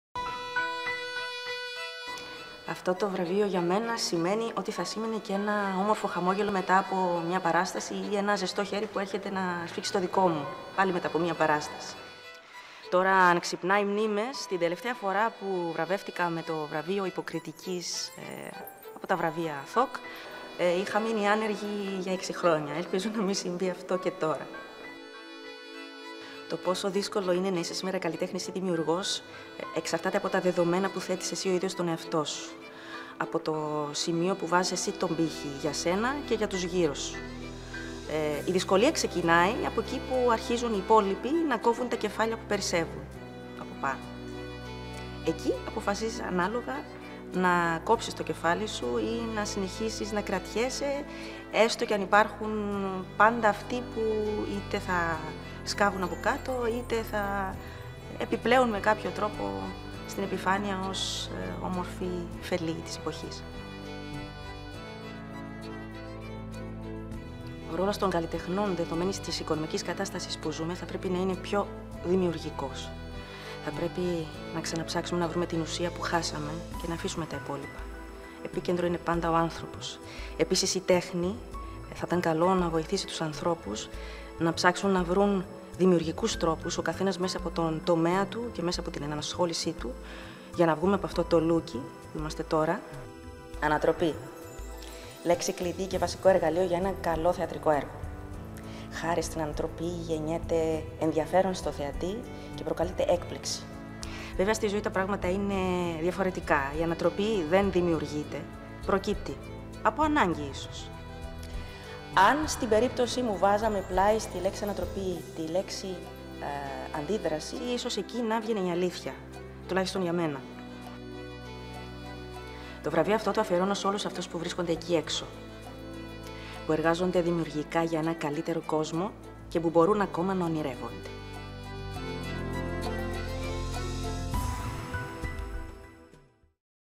ilovestyle.com - Λέα Μαλένη: Δημιουργός Γυναίκα της Χρονιάς 2012
Λέα Μαλένη: « Την τελευταία φορά που πήρα βραβείο είχα μείνει άνεργη για έξι χρόνια»Τι δήλωσε η νικήτρια των Βραβείων Madame Figaro Γυναίκες της χρονιάς 2012, στην κατηγορία Δημιουργός Λέα Μαλένη.